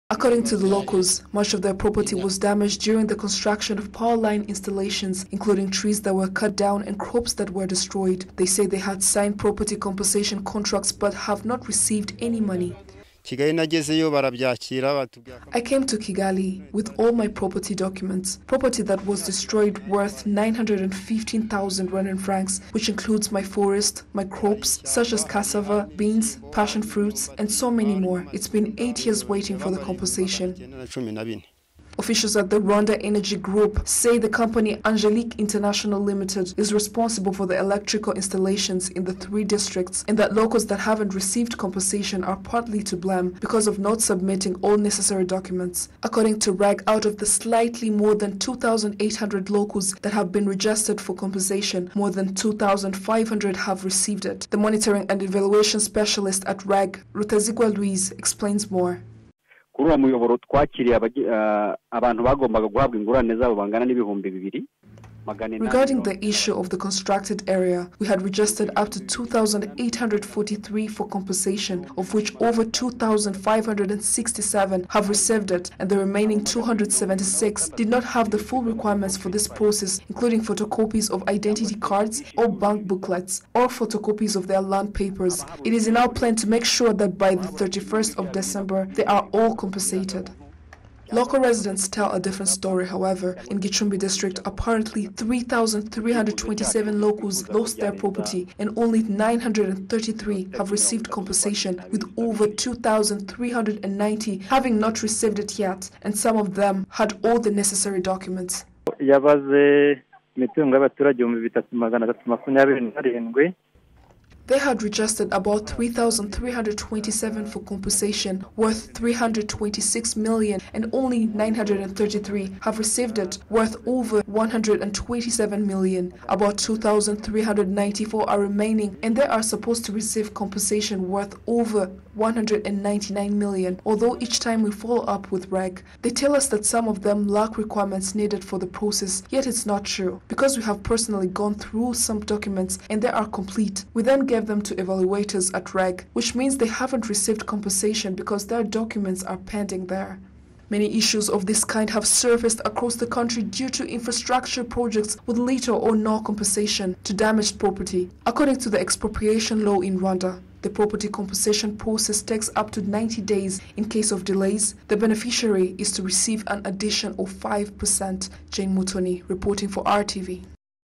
2300 citizen of Northern Province decry 8-year delayed land compensation from REG
After 8 years of uncompensated properties belonging to the locals in Gicumbi, Rulindo and Burera Districts that were damaged due to the construction of power line installations by the Rwanda Energy Group, over 2,300 locals are complaining about this issue but the Rwanda Energy Group claims only 250 haven’t received compensation.